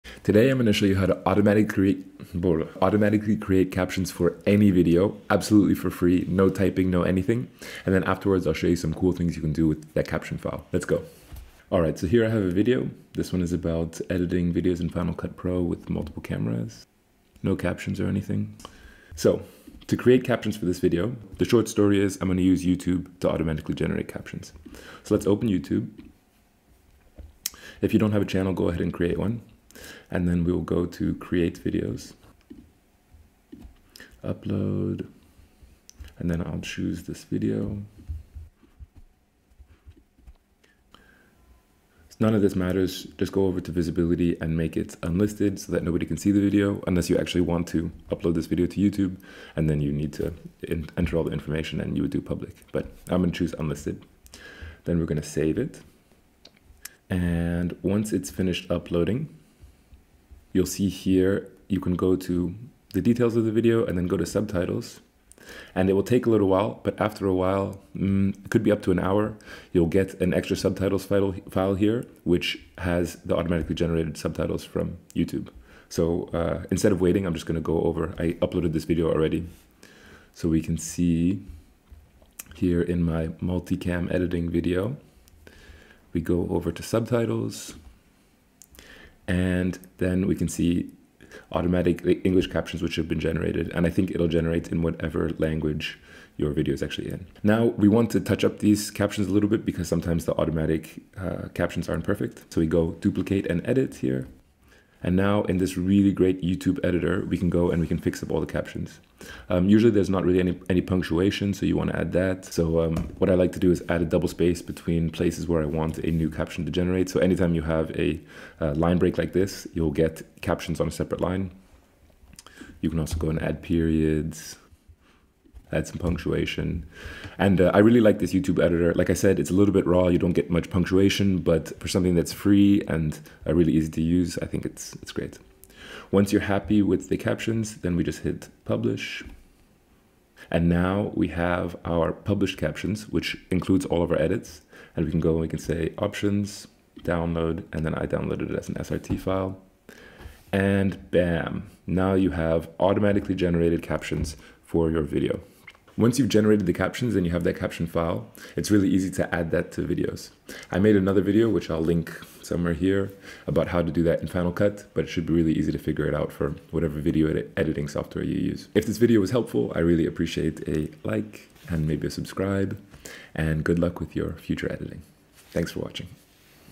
create automatic captions for any video
this is how i caption all of my edited videos for free.

i made another video about how to take the caption file and apply some styling in final cut pro, see the video

if you just want to quickly add captions to a video on your phone, i suggest captions app (it costs $40 but it is fantastic)